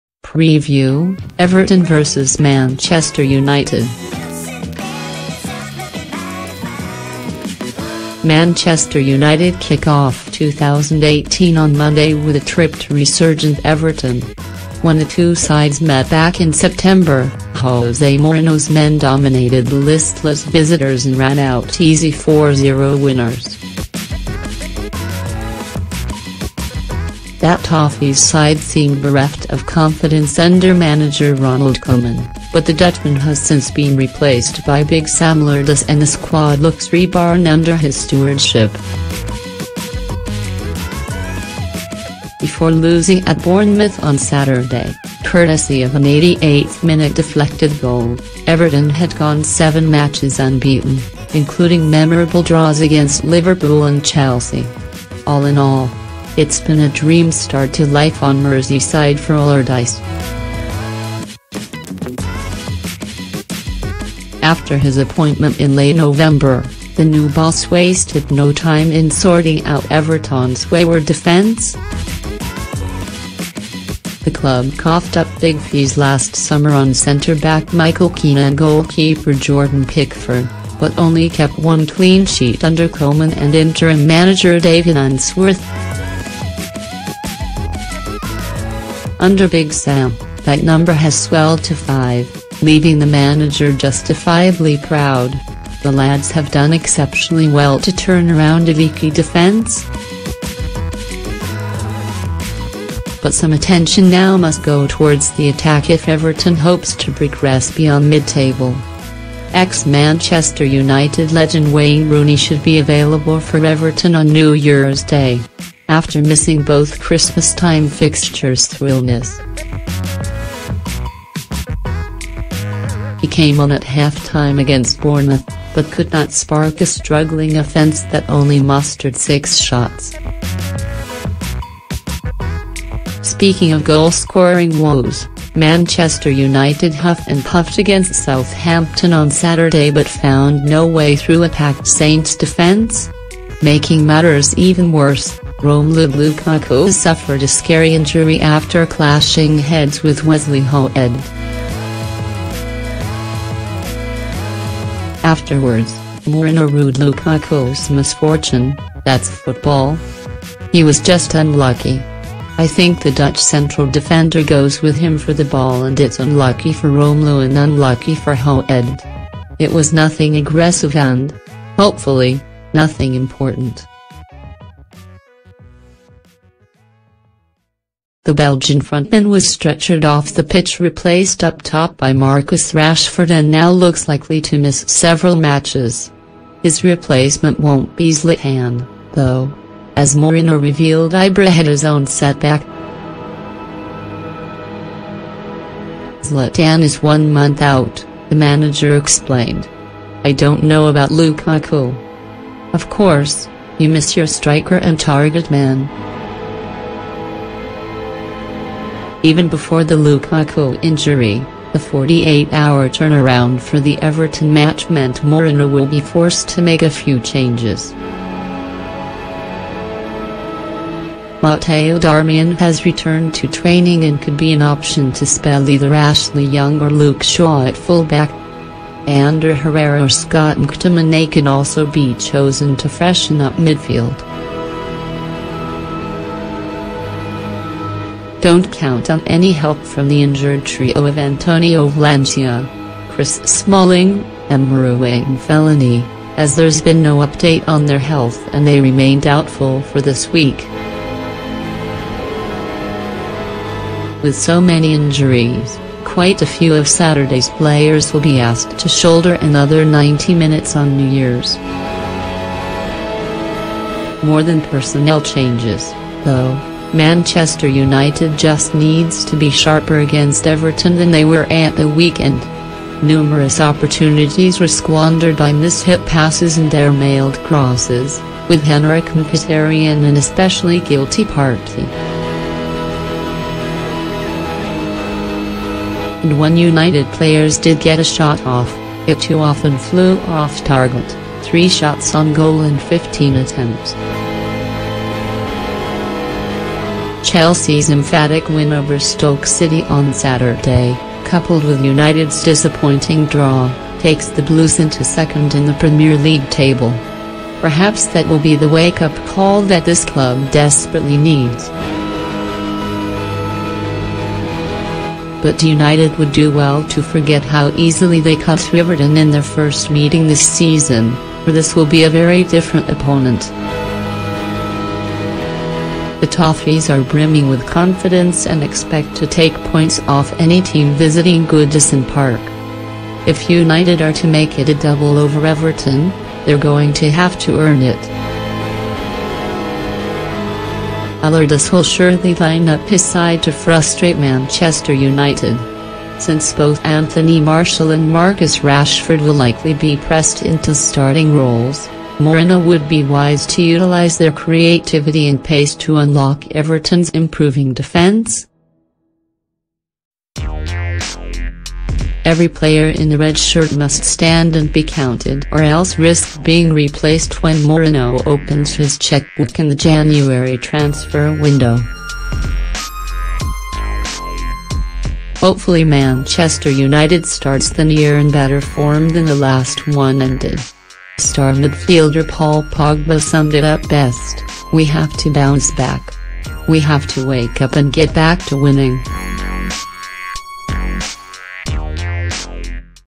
Preview: Everton vs. Manchester United
United kick off 2018 with a trip to Everton and a reunion with Wayne Rooney.